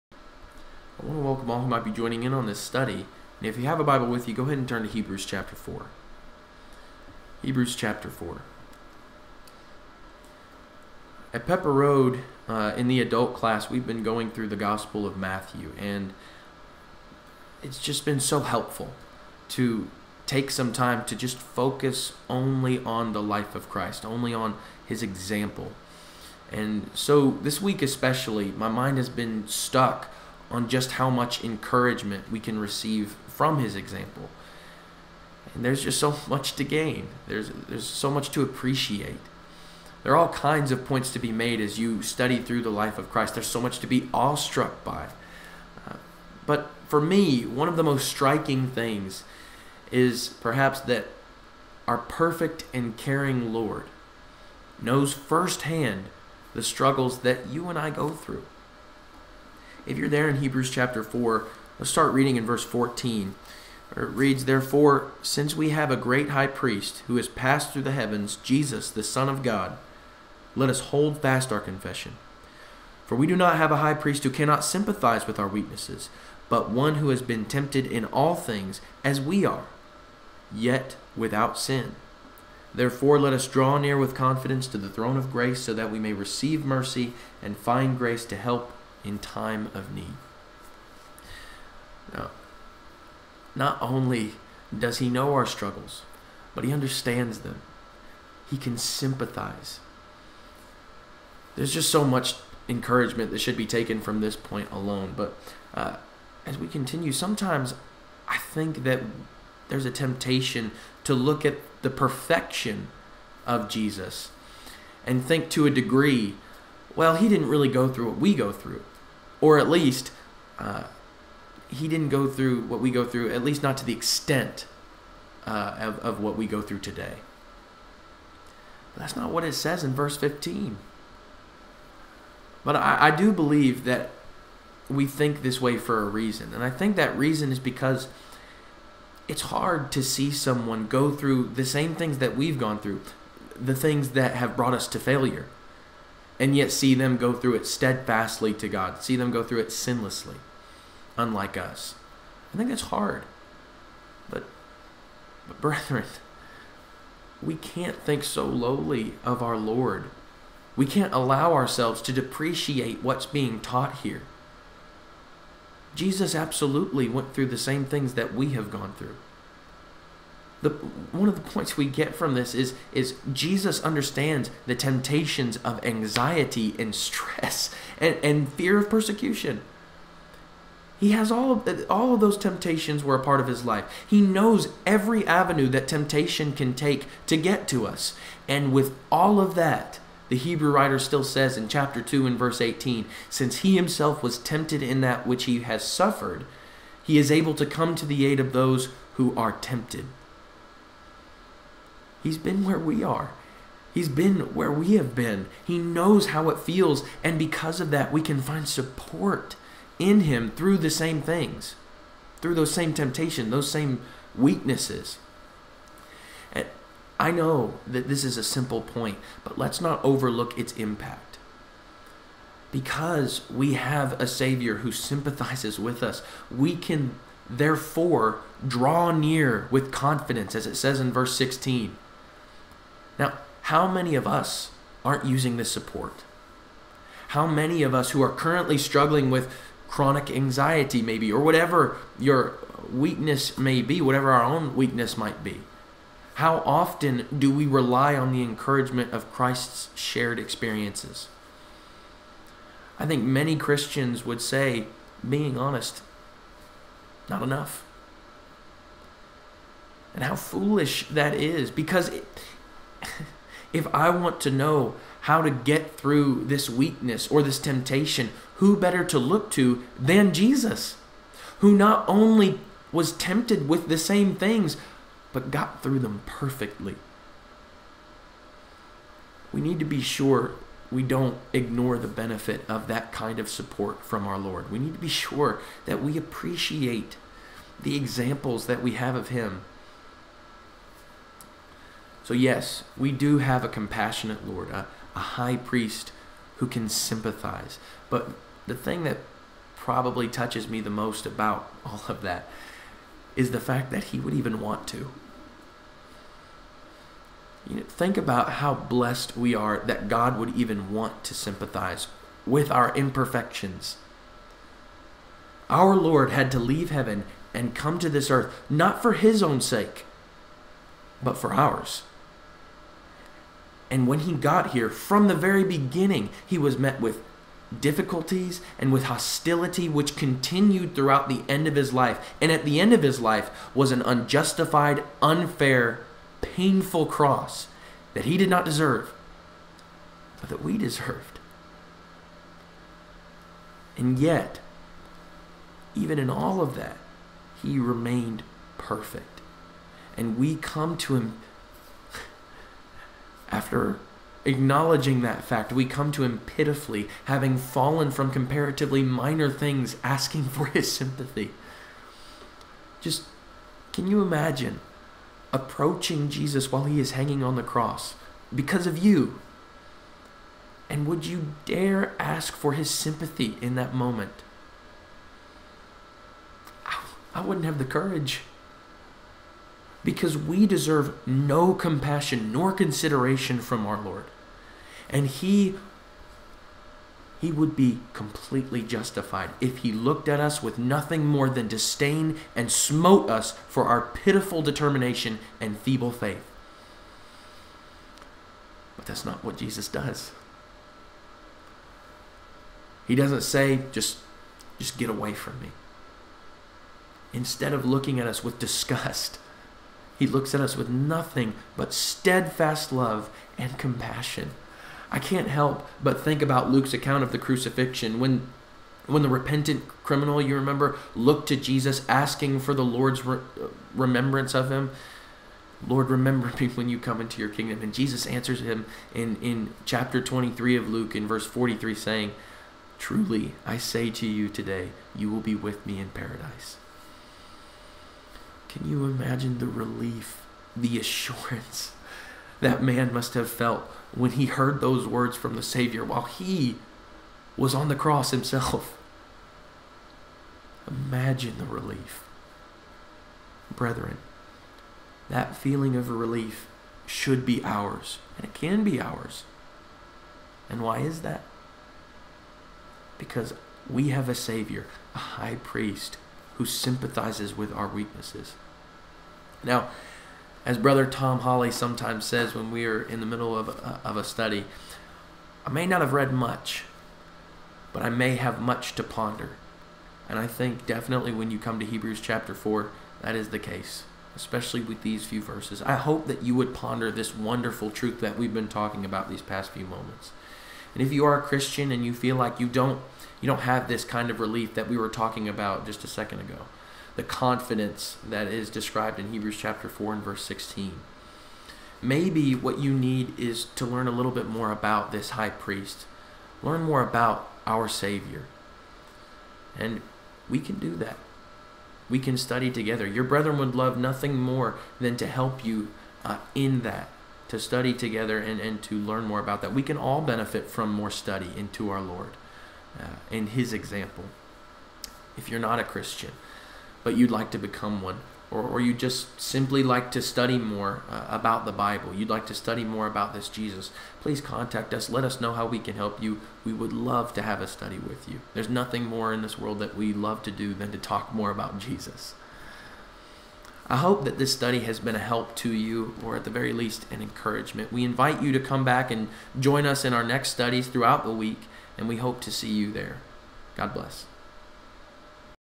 A Savior Who Can Sympathize
Our Lord lived a perfect, sinless, infallible life while He was on this earth. What great comfort we should receive when thinking about how He showed, and still shows today, compassion for those who were imperfect.